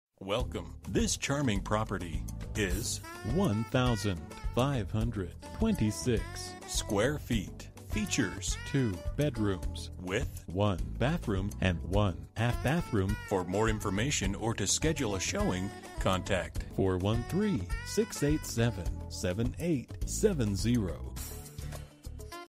Property for sale - 9 Castlegate 9, Springfield, MA 01129
Come see a Townhouse Condo. offering updates at a great value! This would require a cash buyer! This two bedroom, two level unit features ceramic tile and hardwood flooring, stone tile backsplash, and granite counter tops in the kitchen. There's central air-conditioning, second floor  (in-unit) laundry, jacuzzi tub, Master Bedroom with walk-in closet and a screened slider to let the air in! There's even more room with the partially finished basement which could be used as a den or office space.

Bedrooms: 2
Bathrooms: 1 full and 1 half bathrooms
Square Feet: 1526
Price: $89,000

09/12/2015 03:16:02 am

Last modified: